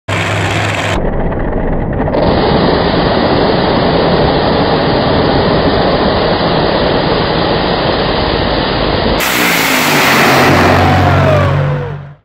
Mile High Nationals - Nitro Qualifiers
Nitro qualifiers going down Bandimere.